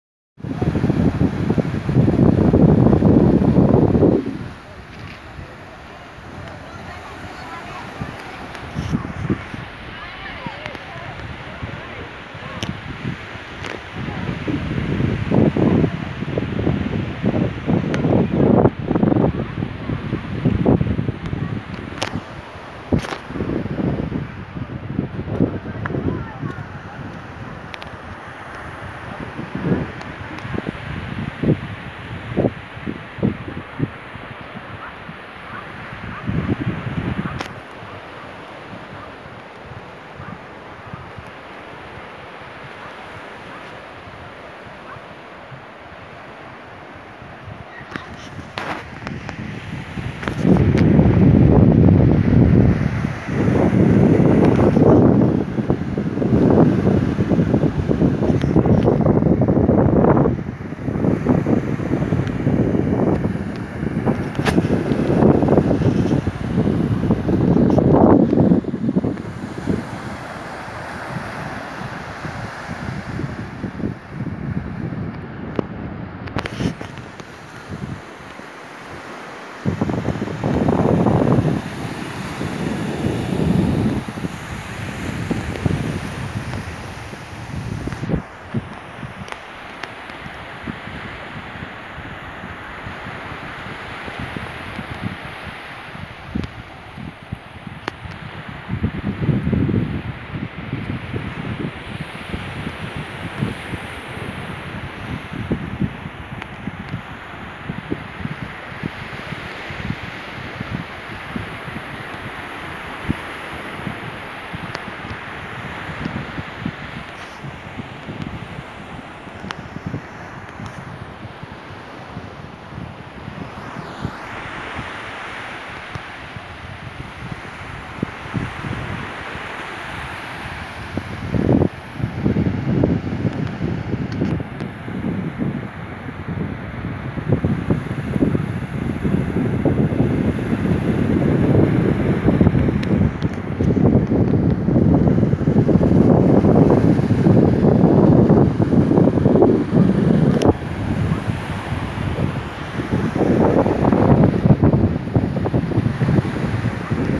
Praia do poço
Poço é uma praia do município de Cabedelo, estado brasileiro da Paraíba. Localiza-se a menos de 20 quilômetros do Centro de João Pessoa, capital do estado, e é vizinha à praia de Intermares. A praia é uma das preferidas pelas famílias da capital para temporada de veraneio.